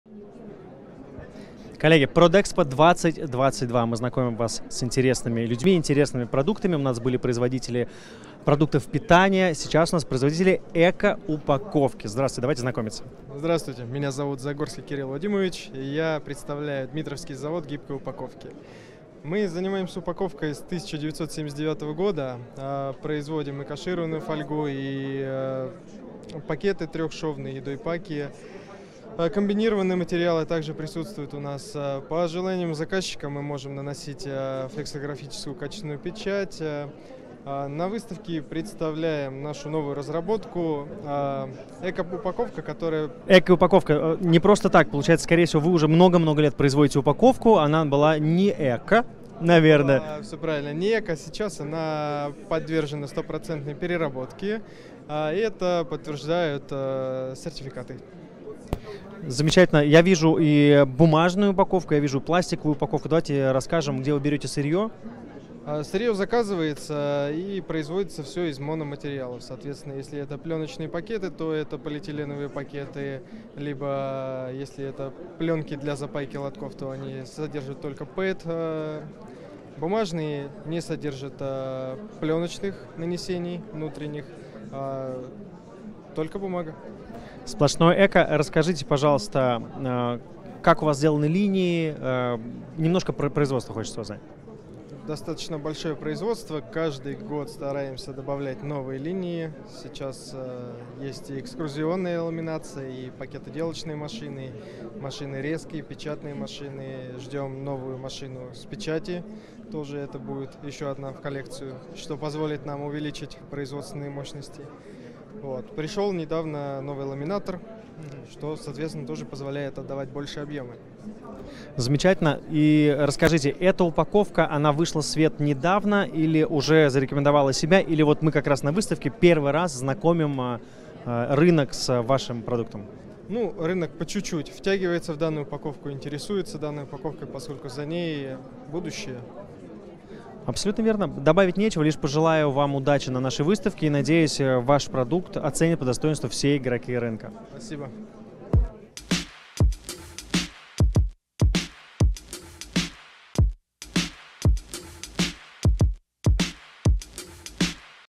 Кирилл Загорский - Дмитровский завод гибкой упаковки на
Кирилл Загорский, Дмитровский завод гибкой упаковки, о новой эко упаковке и ассортименте, а также истории завода